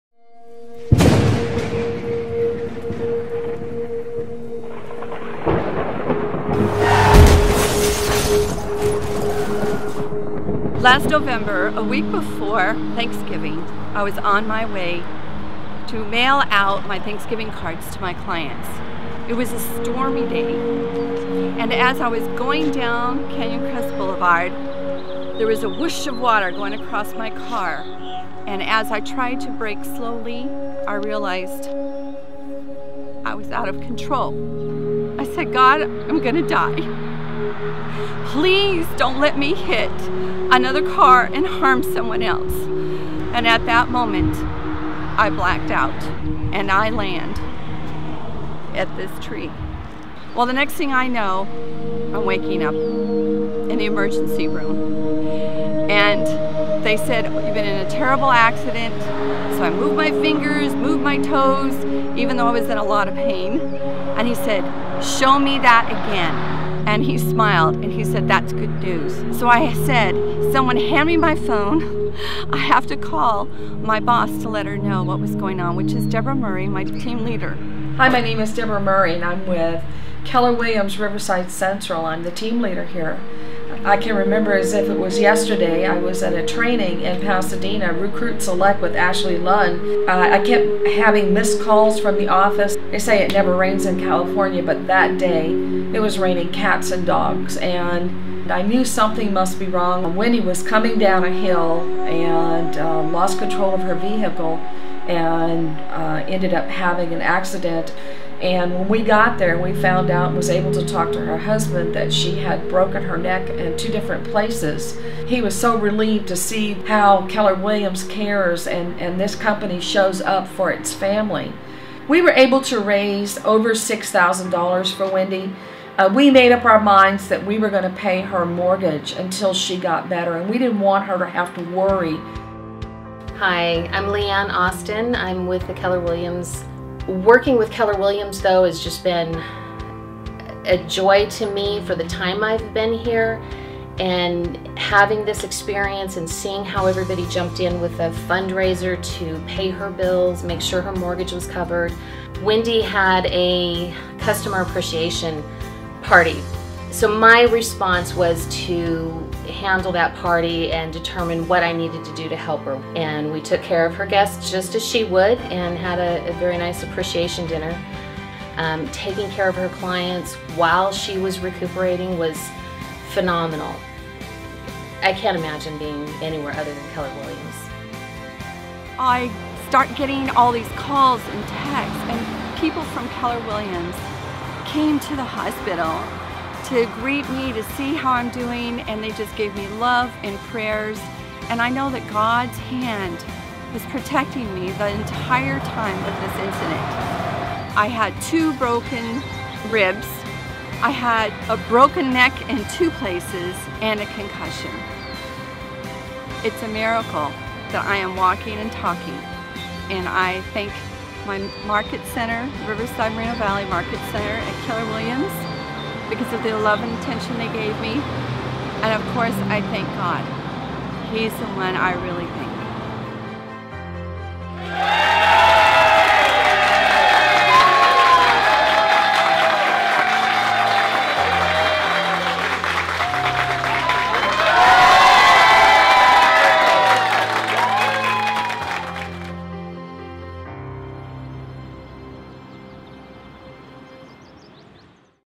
WINDY'S INSPIRATIONAL STORY of Surviving A Broken Neck
Windy's inspirational story of survival
is really about YOU Keller Williams Riverside/Moreno Valley Market Center.

YOU have demonstrated that
Windy's life is worth fighting for!
Her real estate business is worth fighting for!
And, our Keller Williams culture is worth fighting for!

Your acts of kindness,
good deeds,
extra effort,
and
your special care of Windy
are evidence of our Keller Williams culture in action!

We are sincerely grateful to you
and deeply inspired by you.
 Thank You ! Every single one of you in Keller Williams Riverside/Moreno Valley Office.